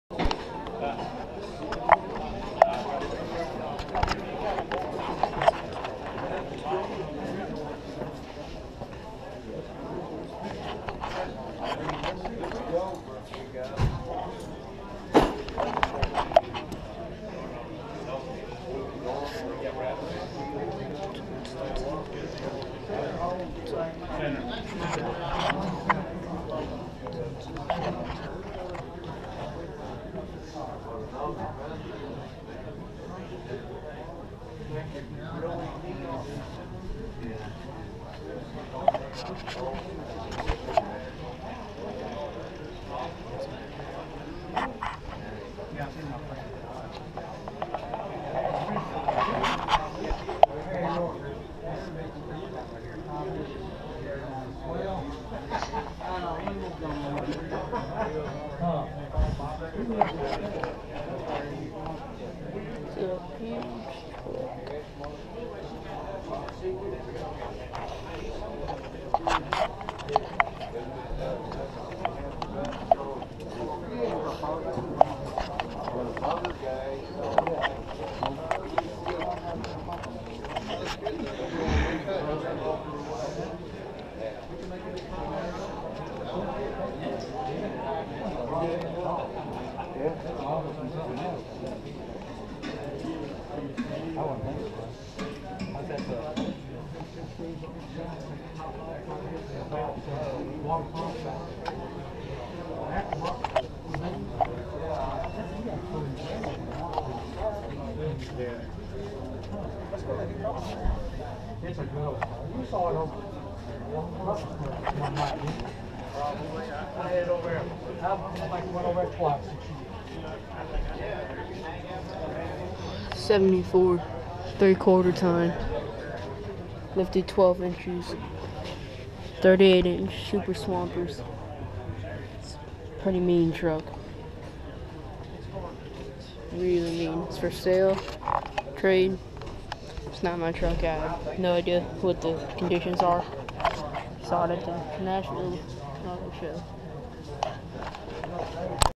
HUGE Lifted 74 GMC K1500
This is the biggest and baddest 3/4 ton GMC I have ever seen Its huge and stands out. Seen at the Nashville Auto Show. Shot on a GoPro Hero 3 White Edition